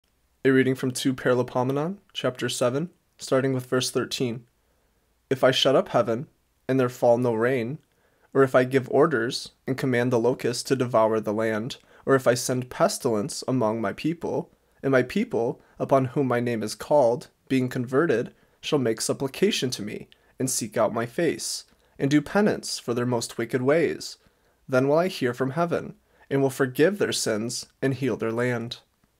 2 Paralipomenon Chapter 7 verses 13 & 14 - He COULD Heal our land...
7:13. If I shut up heaven, and there fall no rain, or if I give orders, and command the locust to devour the land, or if I send pestilence among my people:
7:14. And my people, upon whom my name is called, being converted, shall make supplication to me, and seek out my face, and do penance for their most wicked ways: then will I hear from heaven, and will forgive their sins and will heal their land.